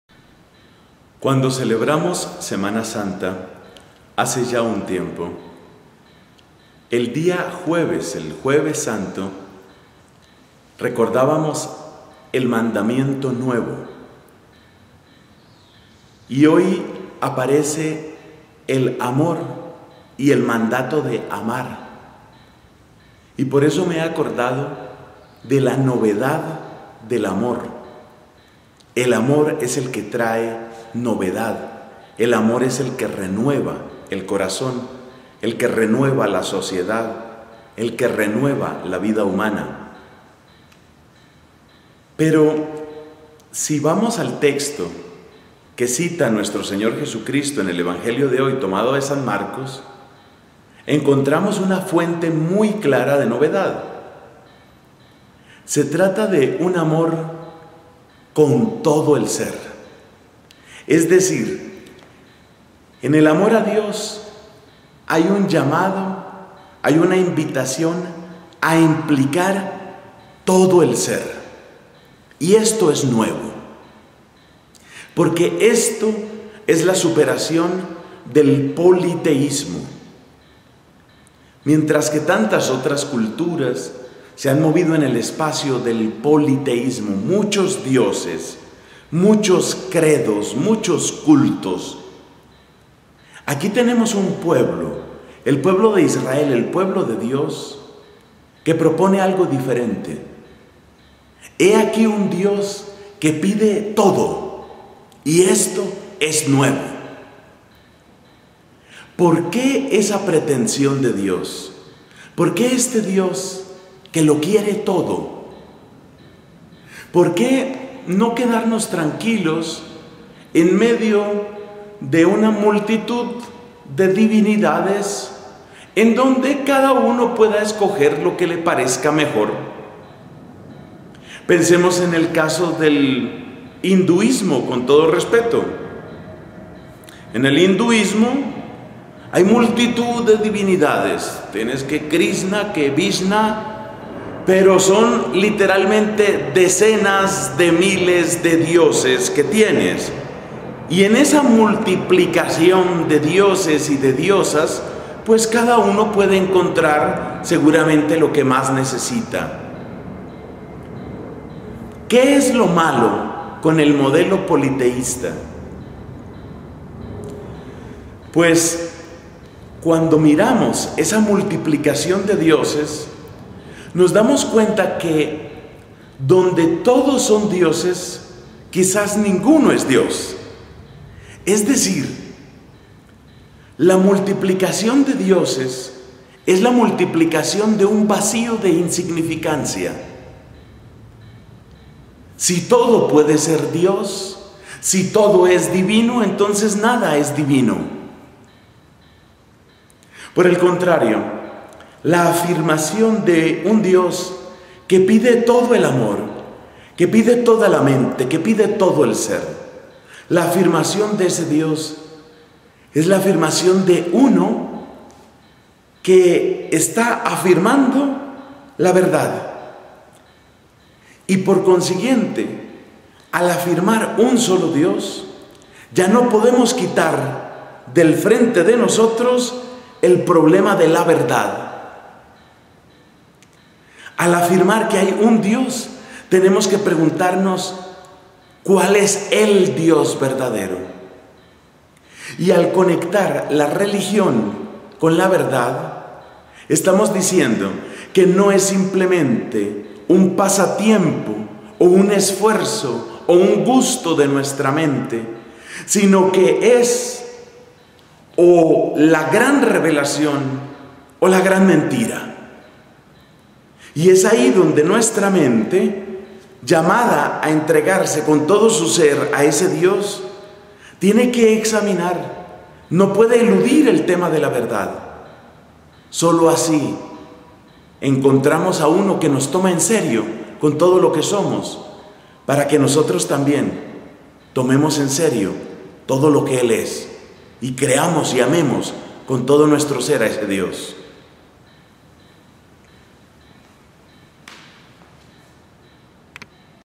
LA GRACIA del Jueves 07 de Junio de 2012
La novedad en el modo de amar a Dios, como lo pide el Deuteronomio, es que este Dios no es uno entre muchos.